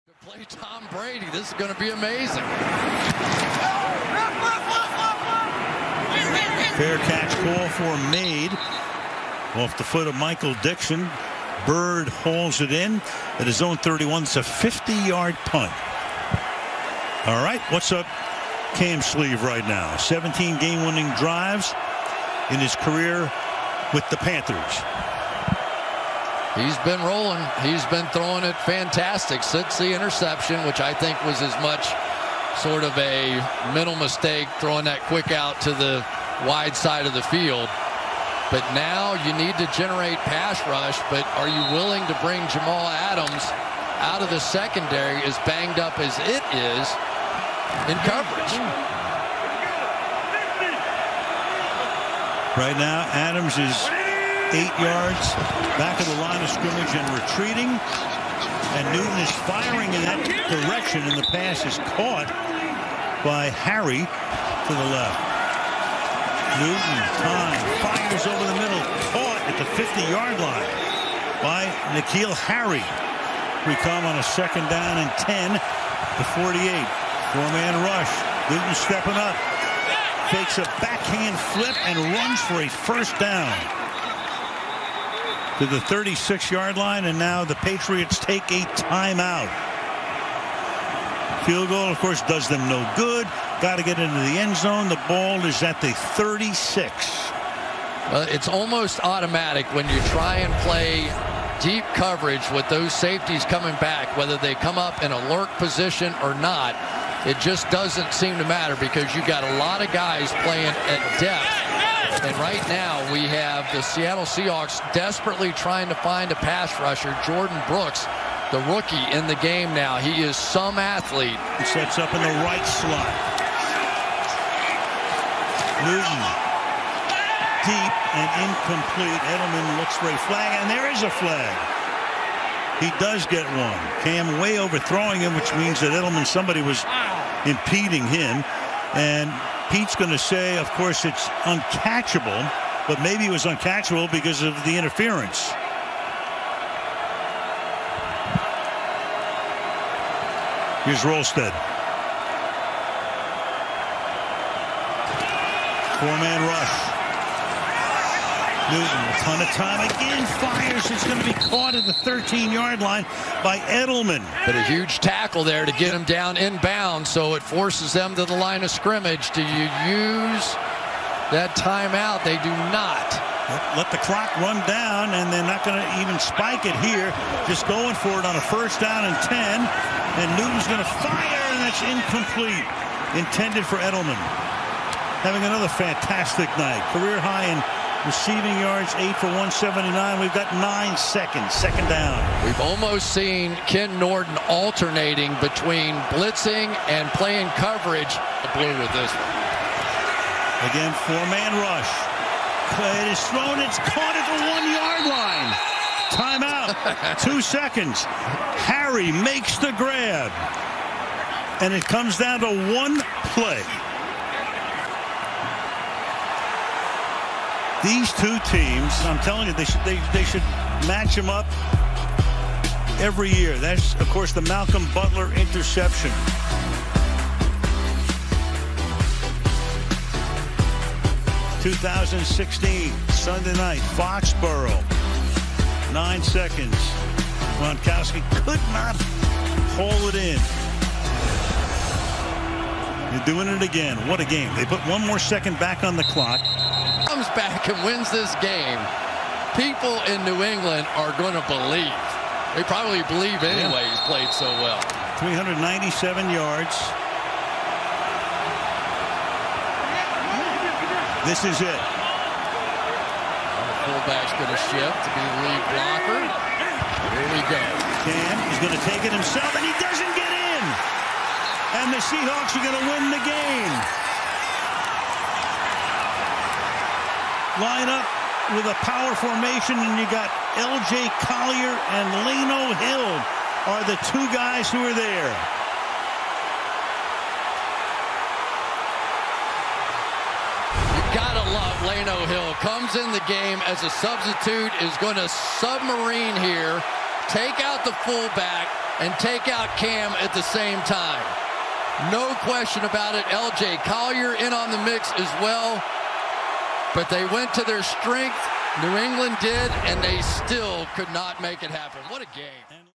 Patriots vs. Seahawks INSANE Ending | NFL 2020
The final drive of one of the best games of the 2020 NFL season.